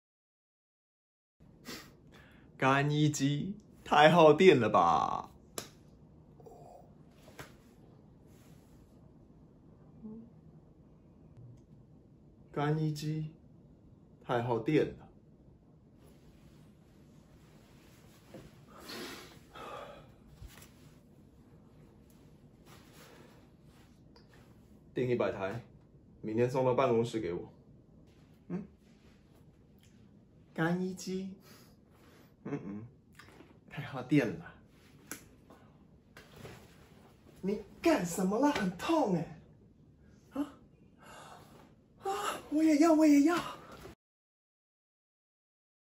乾衣機廣告試鏡帶｜林家偉
我是林家偉 Wesley Lin，劇場演員，擅長當代舞，以音樂劇、台語戲劇為主，也從事影視、廣告拍攝。

● 國語、台語：母語
● 英語：流利
● 日語法語：旅遊會話程度

期待與您合作～